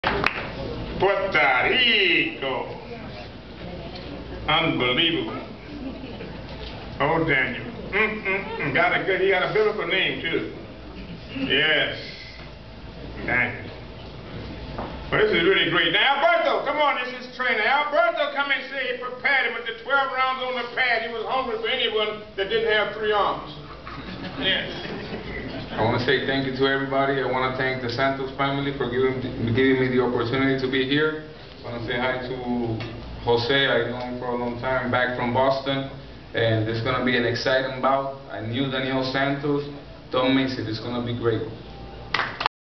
Press Conference with Don King
Press Conference on fight card at Madison Square Garden.